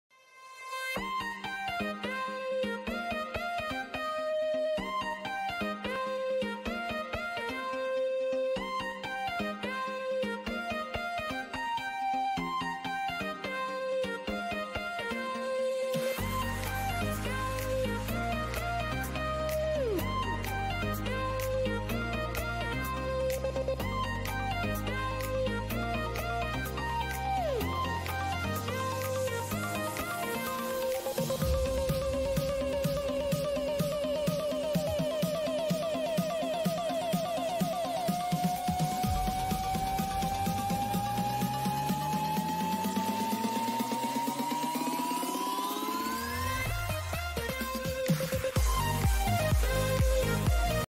LBS650 LBS750 LBS1100 cold laminating machine
Model LBS-650、750
Maximum laminating width 635mm/740mm
Maximum laminating thickness 30MM
machine size 710/810*210*280mm
machine weight 14kg/15kg
upper rubber roller diameter 65mm
bottom rubber roller diameter 65mm
More details contact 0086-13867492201(wechat, whatsApp)